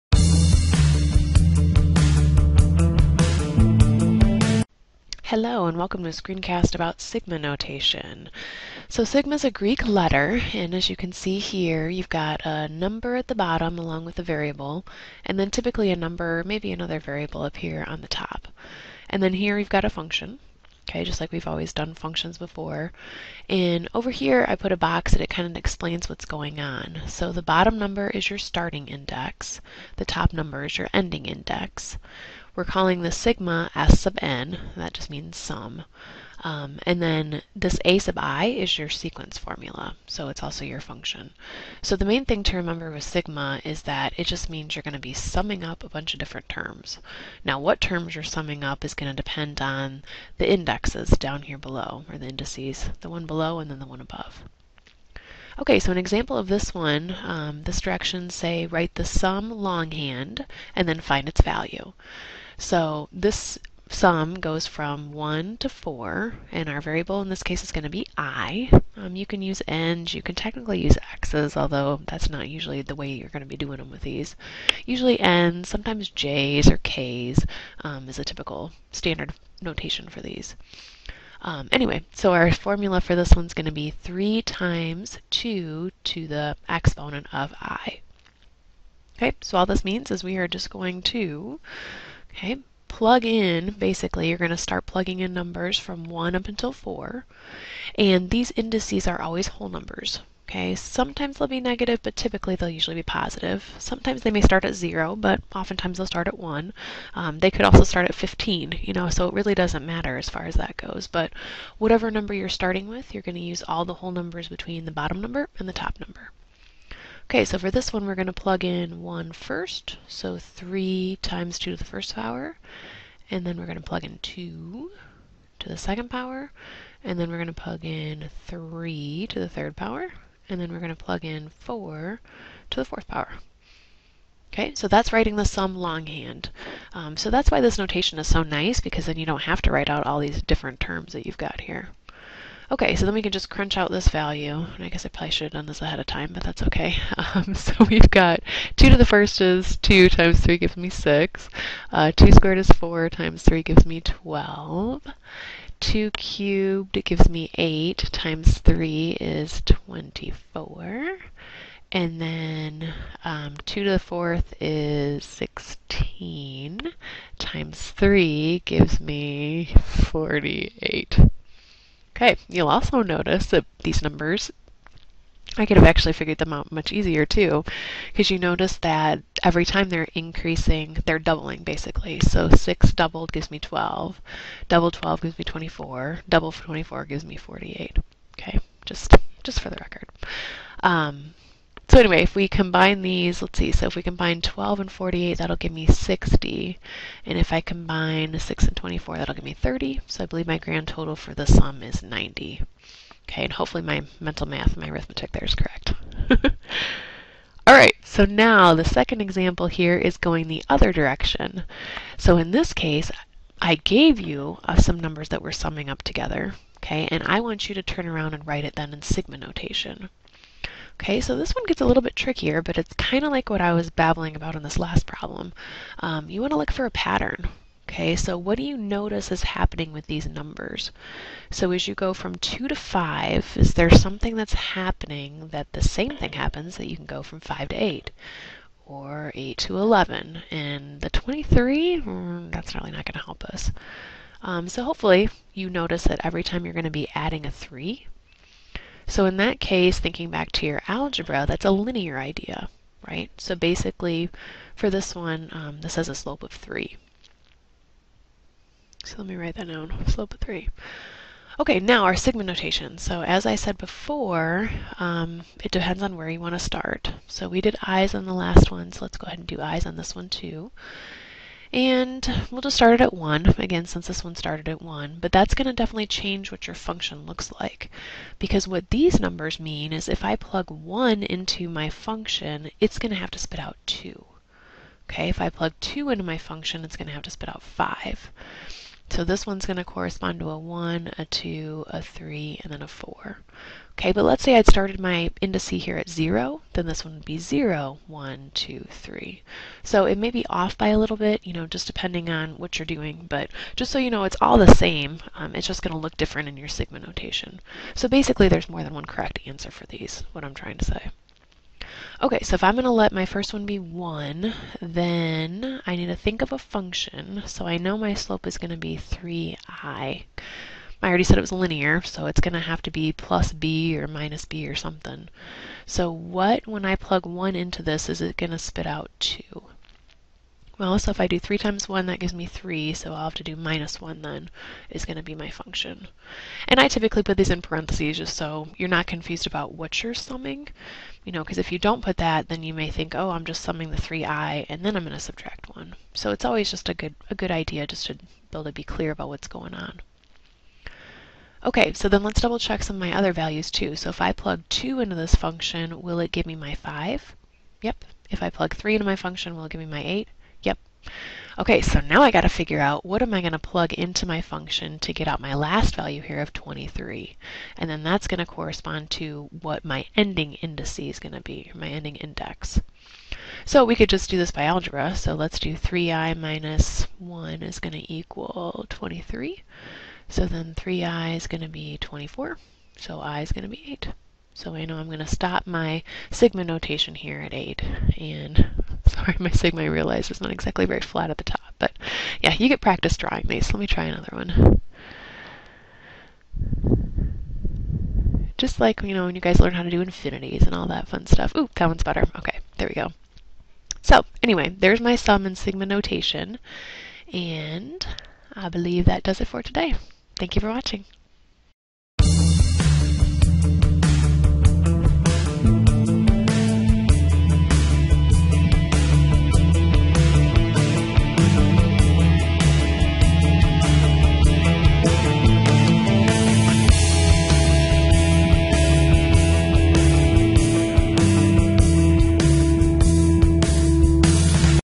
Screencast 4.2.2: Sigma notation
Shows how to use sigma notation to write out a sum as well as how to write a sum with sigma notation.